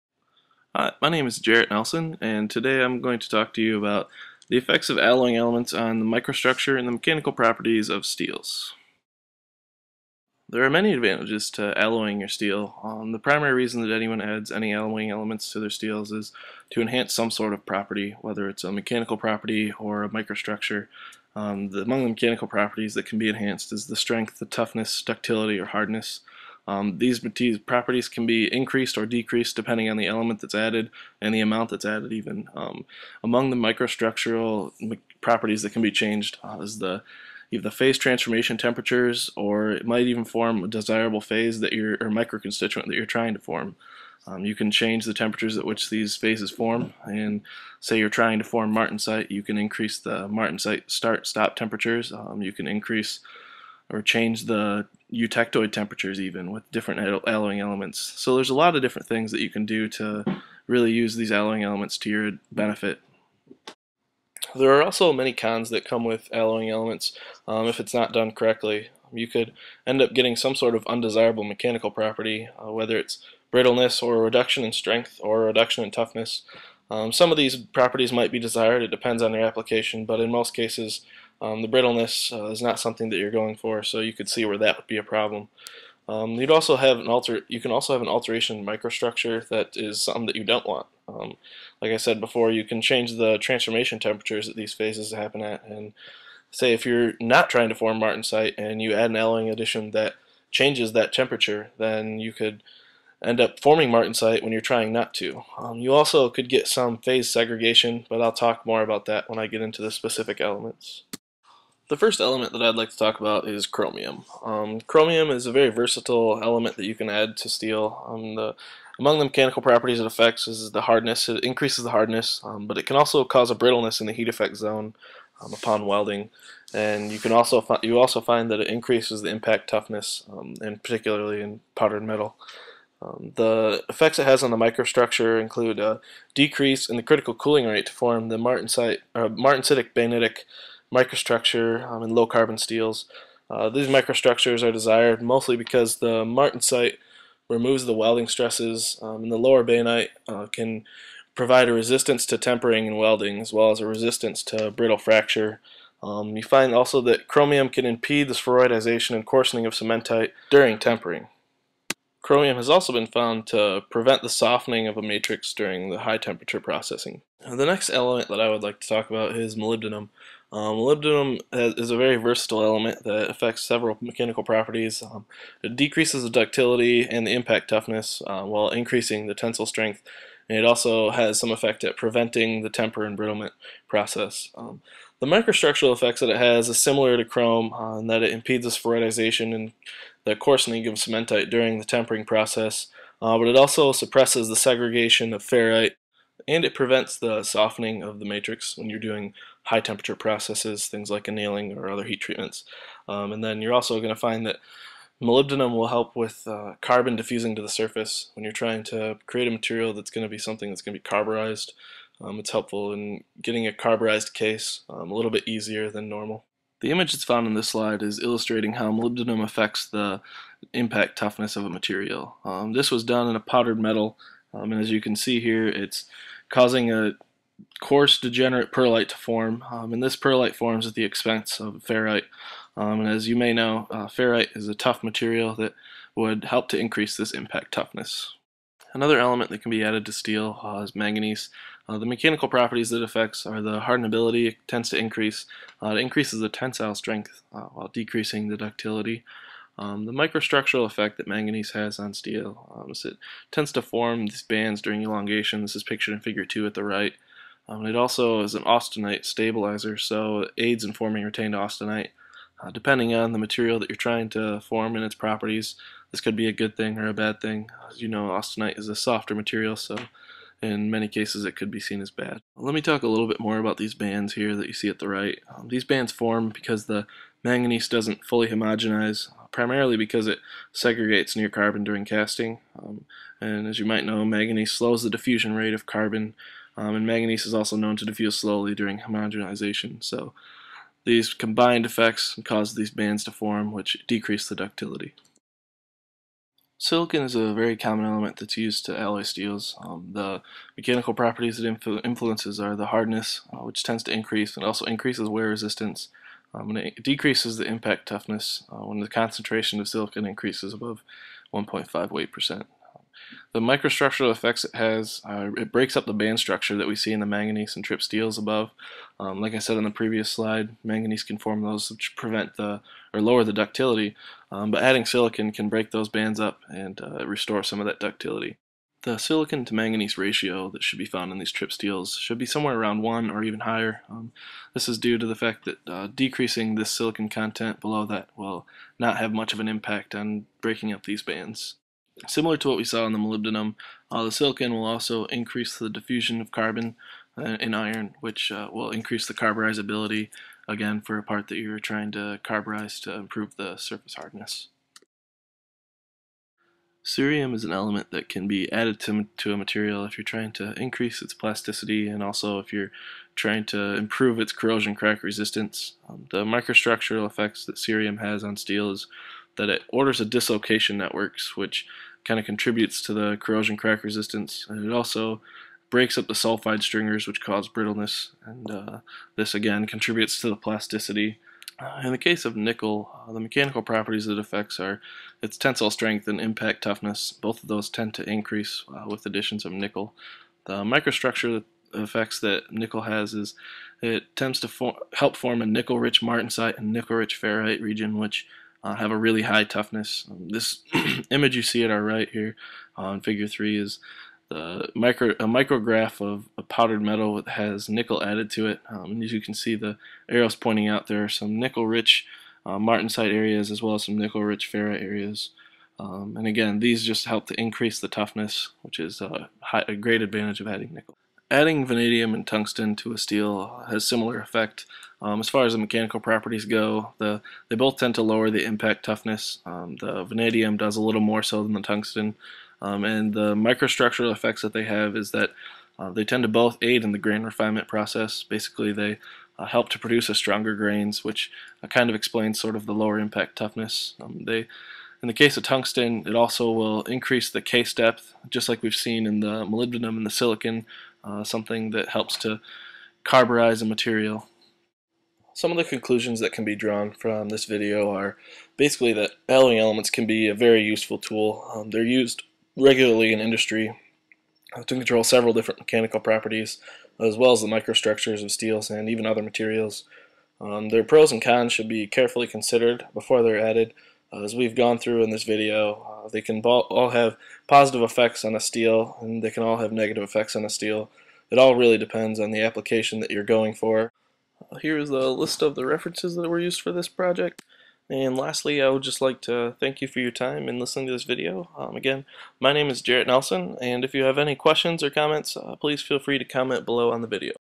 Alloying Elements Presentation
This is a video presentation that was done for my MSE 476 class at MSU. It explores the effects that different alloying elements have on the mechanical properties and microstructure of steel.